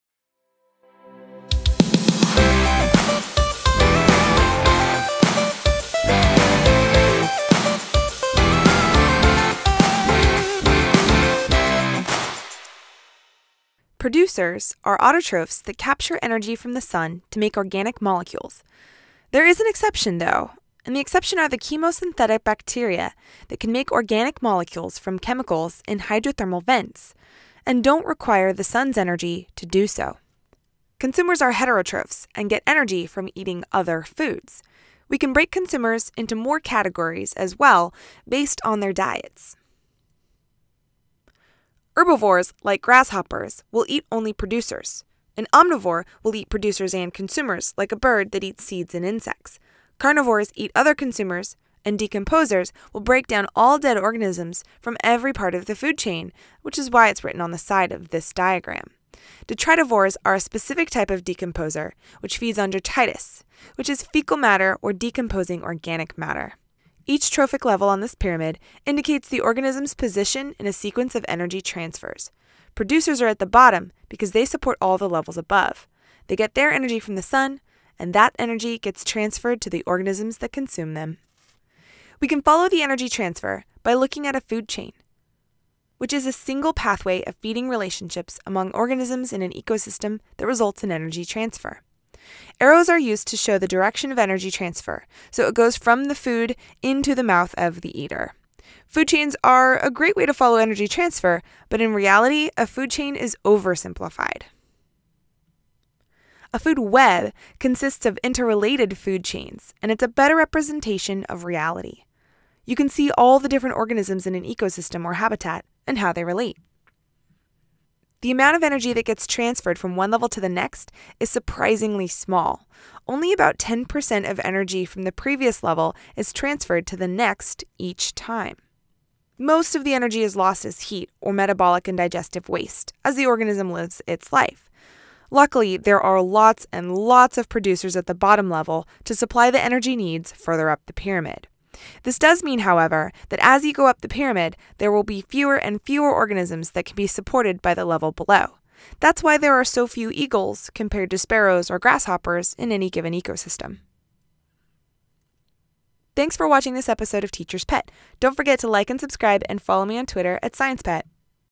Energy Transfer in Trophic Levels
Learn about producers, consumers (herbivores, carnivores and omnivores), decomposers (detritivores), energy transfer and energy loss, and food chains and food webs in this video!